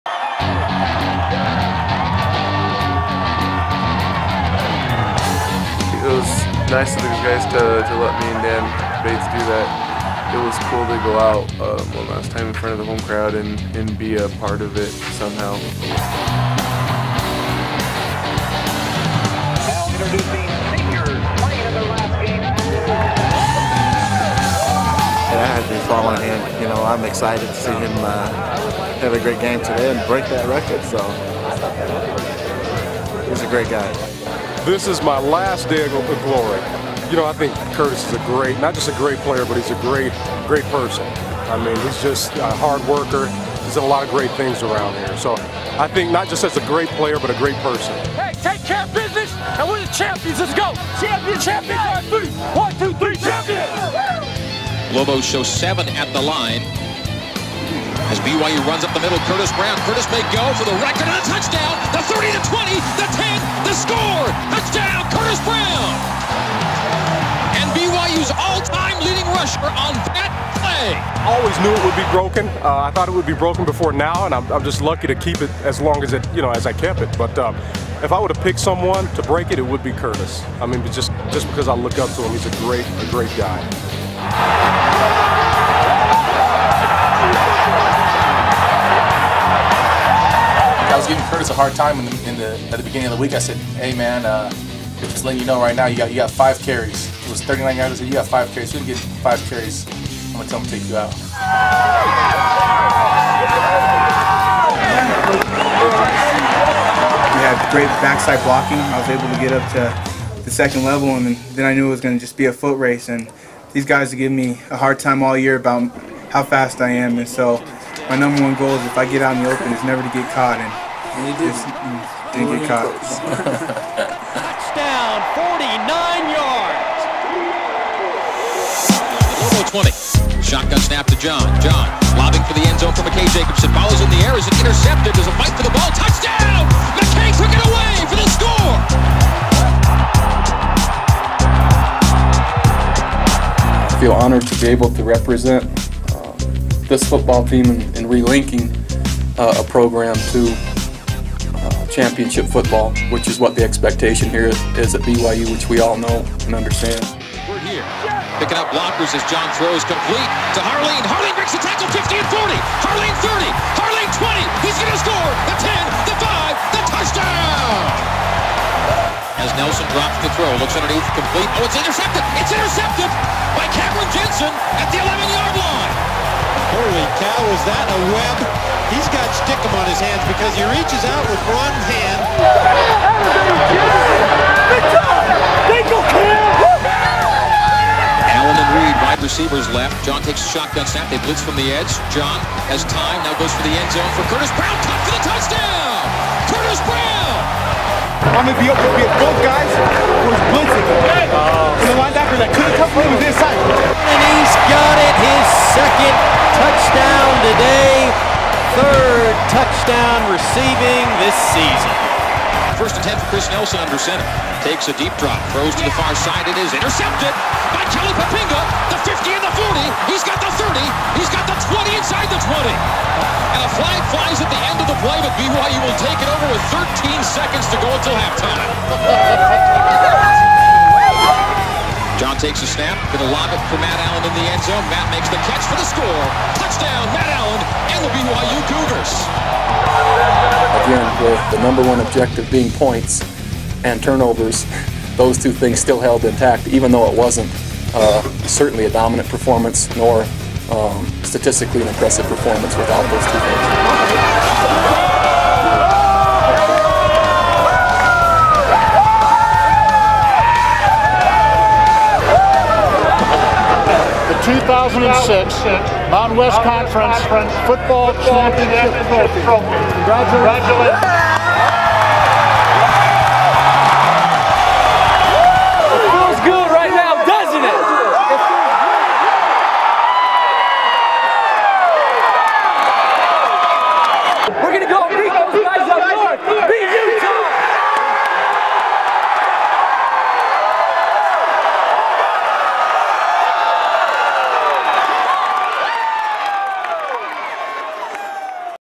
BYU Football - BYU vs New Mexico 2006 - Game 11 (John Beck)
Video taken From BYU's Season in Review DVD...Buy it!  it's awesome